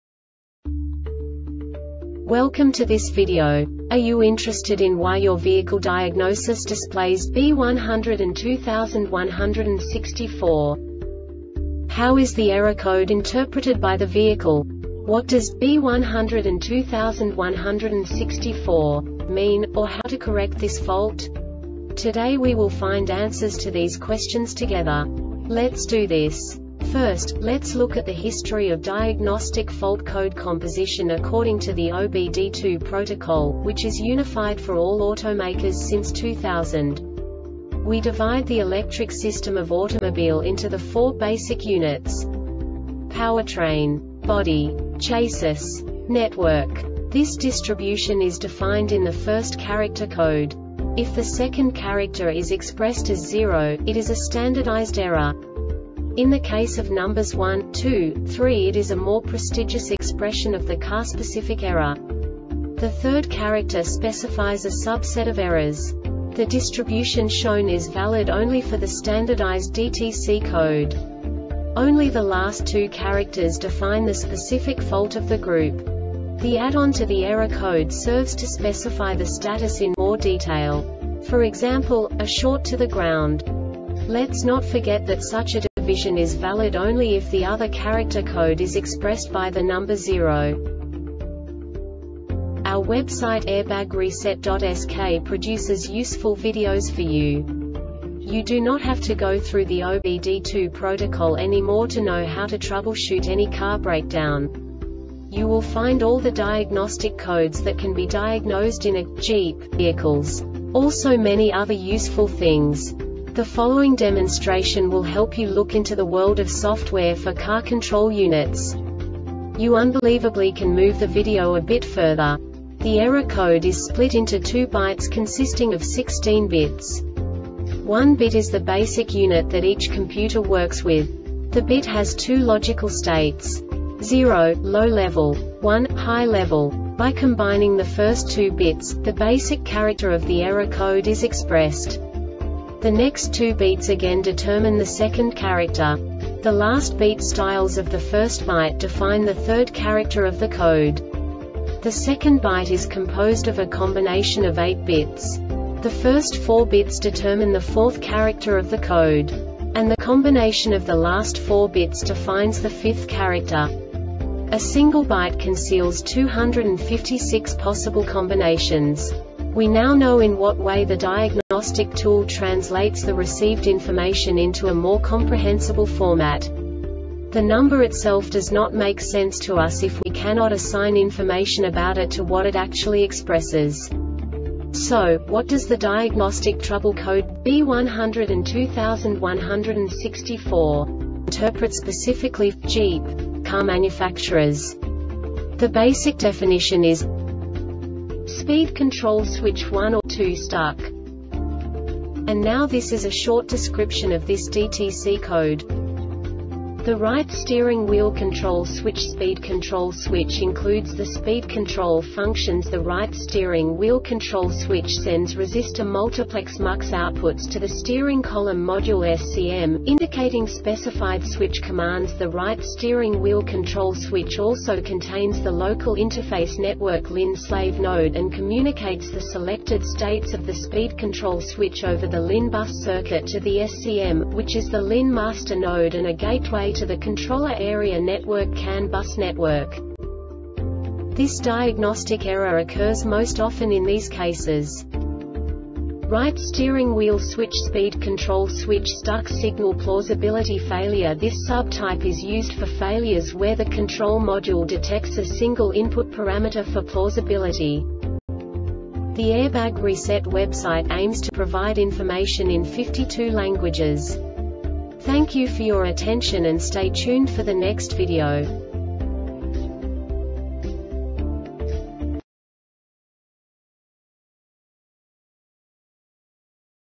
DTC Jeep B1021-64 Short Explanation
Contents:
0:21 Basic DTC analysis according to OBD2 protocol standard.
1:48 Insight into programming
2:58 A diagnostically understandable description of the Diagnostic Trouble Code
3:05 Basic error definition

Make:
Jeep
Code:
B1021 64
Definition:
Speed Control Switch 1 or 2 Stuck
Description:
The Right Steering Wheel Control Switch (Speed Control Switch) includes the Speed Control functions. The Right Steering Wheel Control Switch sends resistor multiplex (MUX) outputs to the Steering Column Module (SCM), indicating specified switch commands. The Right Steering Wheel Control Switch also contains the Local Interface Network (LIN) slave node and communicates the selected states of the Speed Control switch over the LIN Bus circuit to the SCM, which is the LIN master node and a gateway to the Controller Area Network (CAN) Bus network.
Cause:
Right Steering Wheel Switch (Speed Control Switch) Stuck
Failure Type: